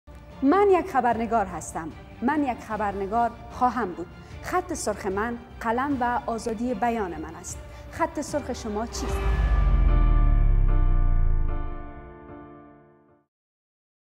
"MyRedLine is my pen!" - Farahnaz Forotan
من یک خبرنگار هستم. من یک خبرنگارخواهم بود. خط سرخ من "قلم" و "آزادی بیان" من است.
خط سرخ شما چیست؟
I am a journalist. I will be a journalist. MyRedLine is my pen and Freedom of Expression. What is your RedLine?